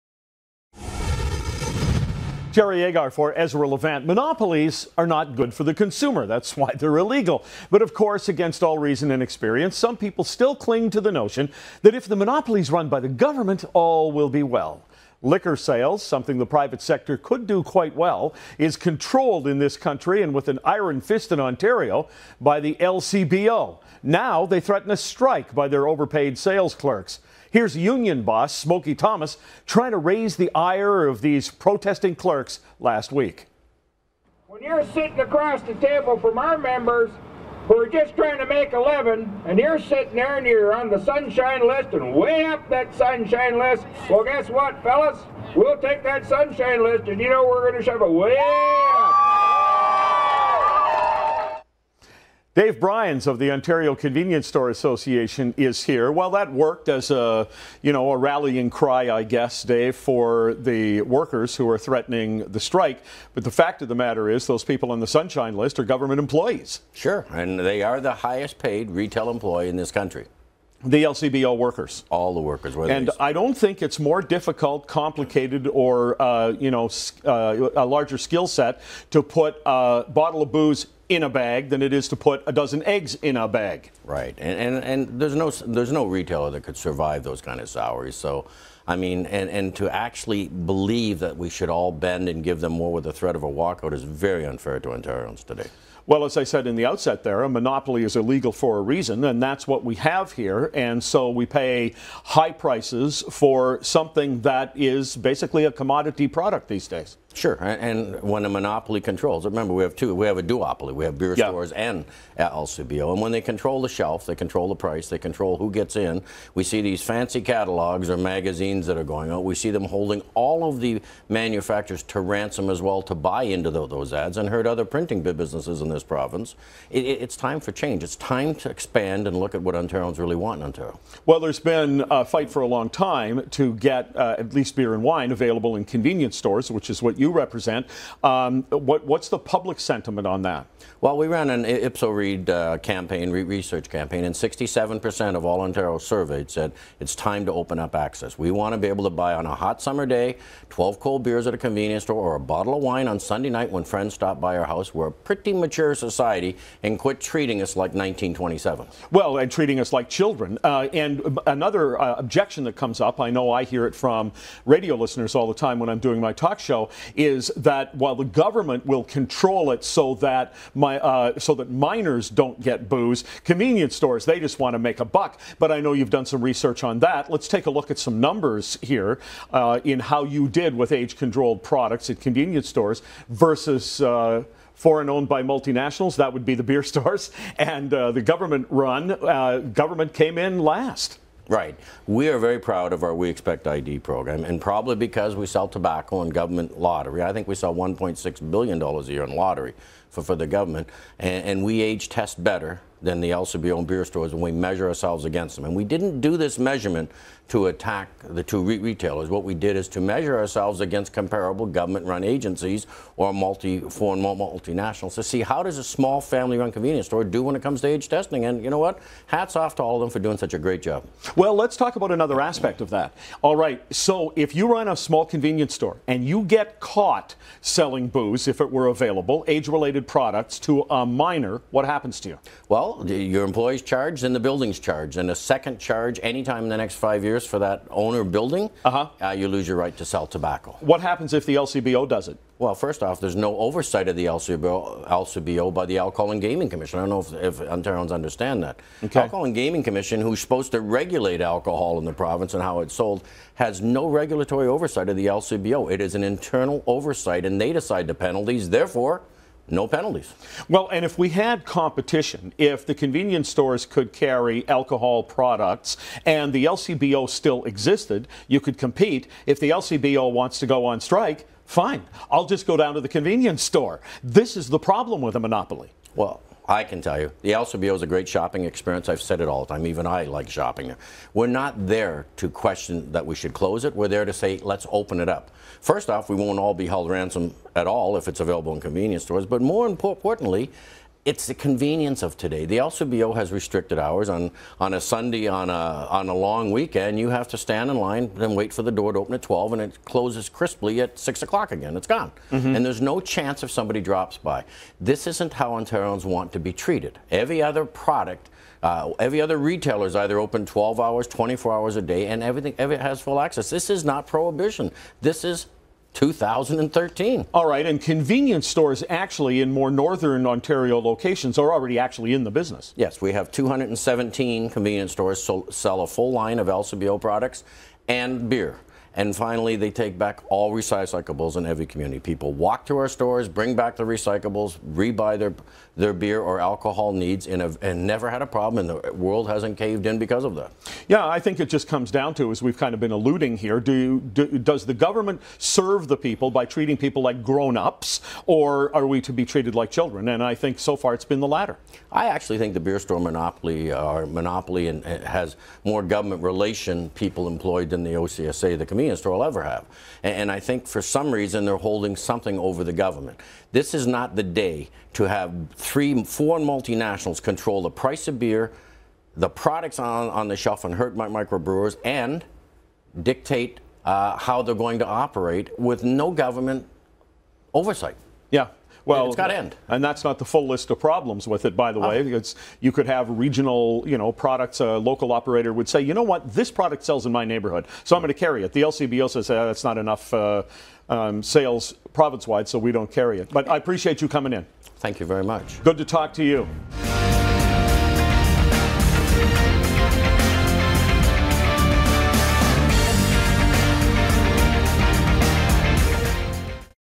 Ontario’s liquor monopoly
Ontario Convenience Stores Association CEO Dave Bryans speaks with Jerry Agar about the overpaid LCBO employees who are threatening a strike.